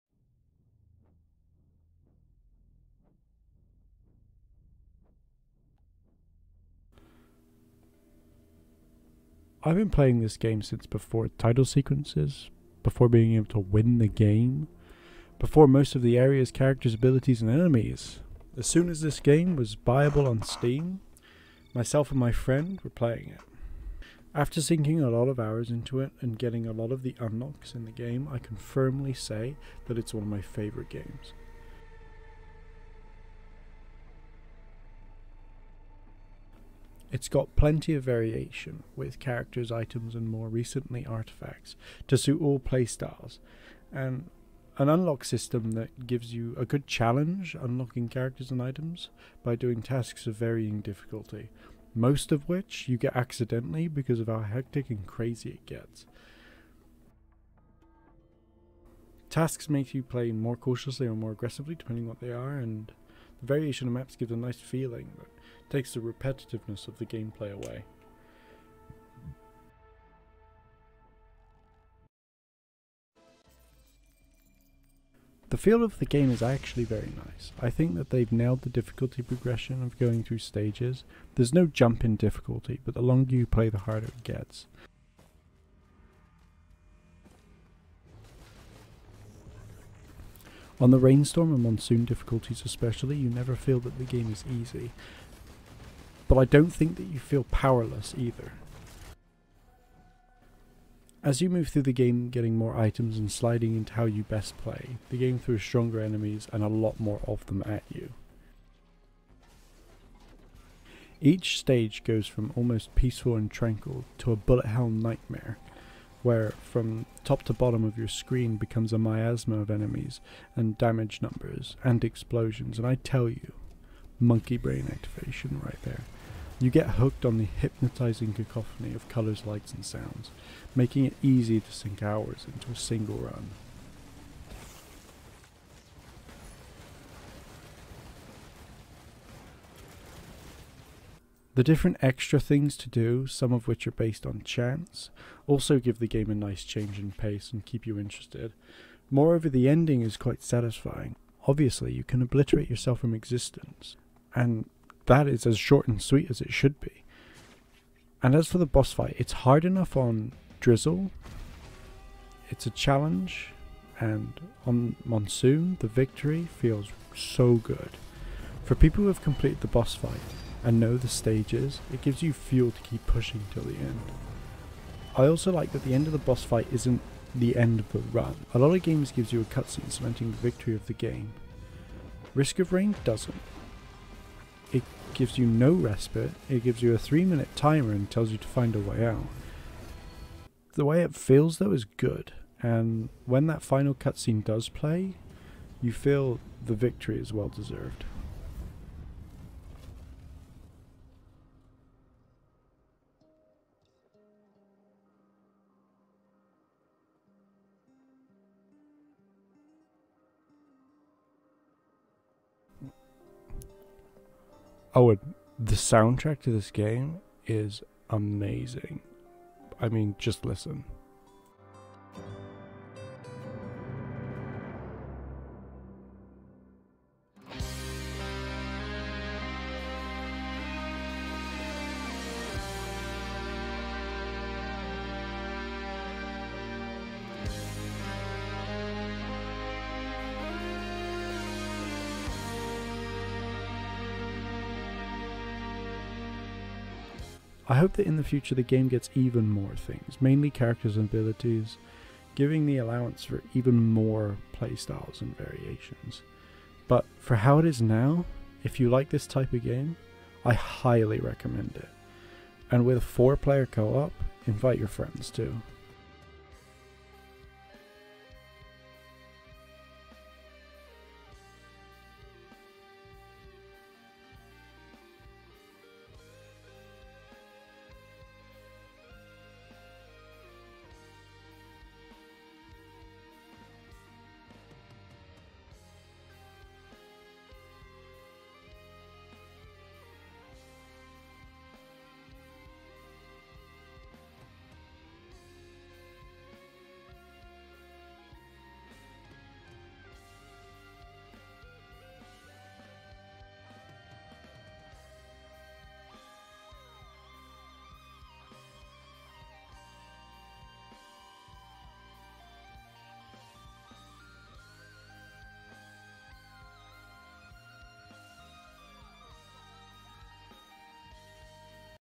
Risk Of Rain2 Review
A honest review of RoR2. a lot of praise for this game but it deserves it.
I am excited to see if there will be any further updates or additions to the game but honestly it doesn't need any.

If you've played RoR2 let me know what you think.